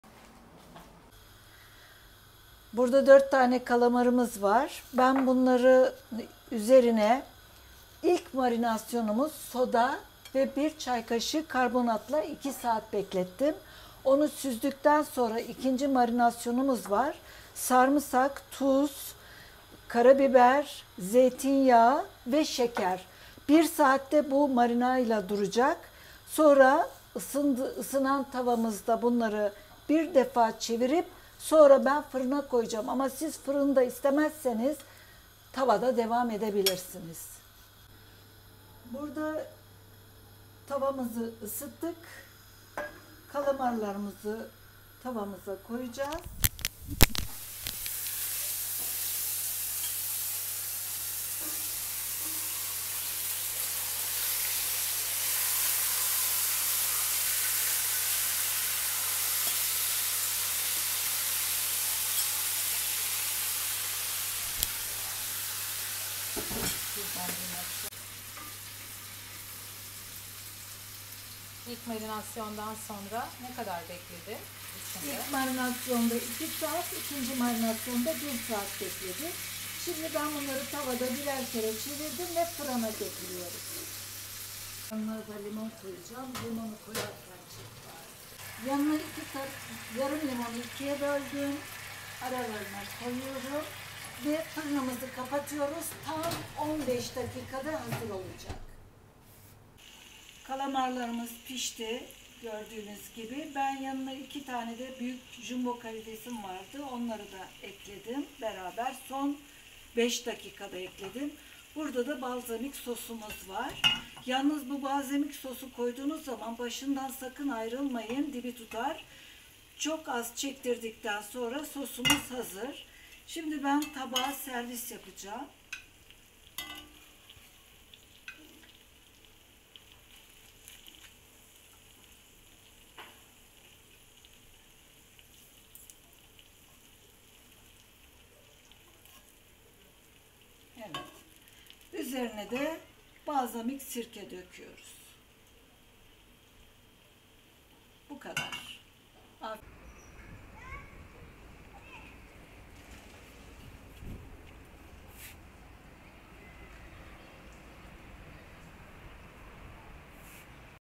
Kalamar tarifi
tarifi  Barcelona dan
Malzemeler:
4 adet kalamar
Birinci marinasyon için;
1 sise soda
1 çay kasigi karbonat
ikinci marinasyon için;
1 çay bardagi zeytinyadi
6 dis sarimsak, ezilmis
1 tatl kasigi tuz

Sos için
1 çay bardagi balzamik sirke
Birinci marinasyonu bir kasede Karistirin ve
temizlenmis kalamarlari marinasyon içerisinde 2
saat bekletin.  ikinci marinasvon malzemelerini bir kasede
karistirin ve birinci marinasyondan çikardiginIz
kalamarlari ikinci marinasyona ekleyin ve 1 saat
bekletin.
Yapılışı
sirke bir sos tavasinda 3 dakika
boyunca kaynatilir ve koyu bir kivam elde edilir.
Marinasyondan çıkardığınız kalamarları bütün
sekilde kızdırılmış döküm tavada iki tarafi 5’er
dakika pisirin sosunu gezdirerek servis edin.😋🙈👌
Çekimi yapan @fazilagulenay teşekkürler canımmm🙏❤️
Canım Oğlum /Mon. fils Pier’e ve kızım figue Figene ayrıca teşekkür ediyorum 😘❤️❤️🙏🙏🫶🏻